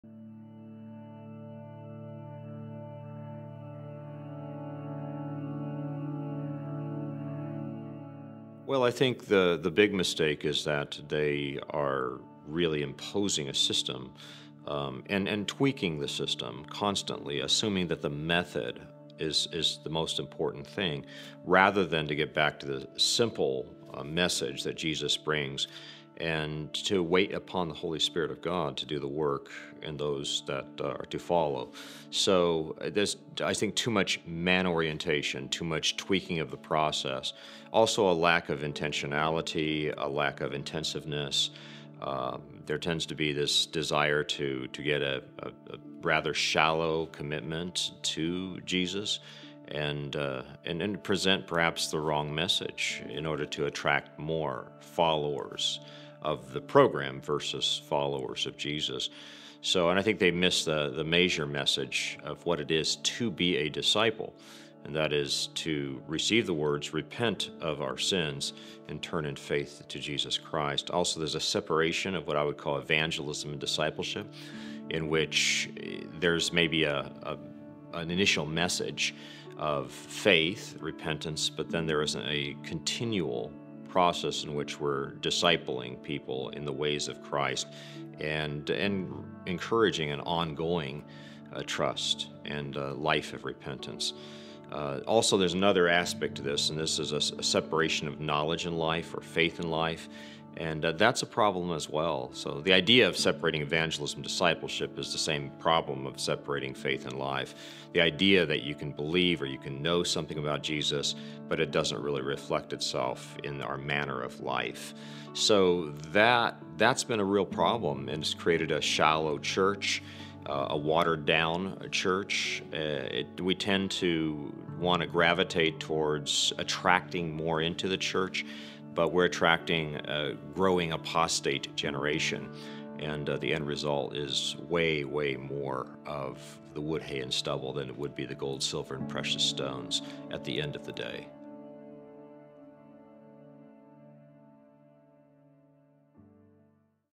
Evangelism and Discipleship
Are we focusing too much on the system of evangelism and missing the simple message that Jesus brings?

Kevin Swanson argues that there's too much human orientation and process tweaking in our approach to evangelism. He raises concerns about a lack of intentionality and intensiveness, leading to a shallow commitment to Jesus. Instead of attracting followers of Jesus, he worries that we are attracting followers of the program. He believes we've strayed from the core message of discipleship, which is to receive the words, repent of our sins, and turn in faith to Jesus Christ.

Swanson also highlights the problematic separation of evangelism and discipleship, and the disconnection between faith and life. He laments that the result is a shallow church that merely attracts a growing apostate generation, yielding more "wood, hay, and stubble" than "gold, silver, and precious stones." He urges a return to a continual process of encouraging an ongoing trust and life of repentance, where faith fully reflects in our manner of life.

2 Timothy 2:15 (NKJV): "Be diligent to present yourself approved to God, a worker who does not need to be ashamed, rightly dividing the word of truth."